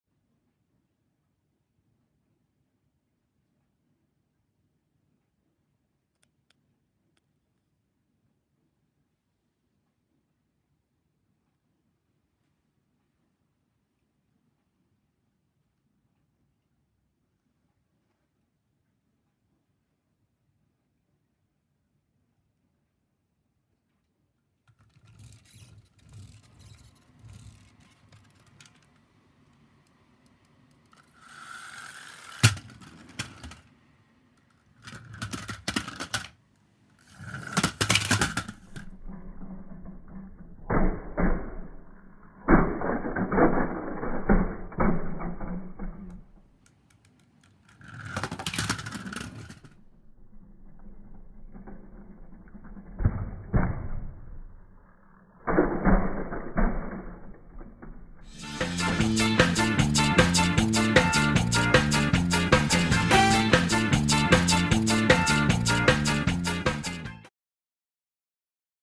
【いんちきおもちゃ動画】爆走！4WDいんちきトーマス！
無軌道に進化を続ける中国のいんちきトーマスが今度は4WDになっていた。
プルバックゼンマイなのに4WD⁉︎
よく見たら商品名が「Thomas 」じゃないし（笑）。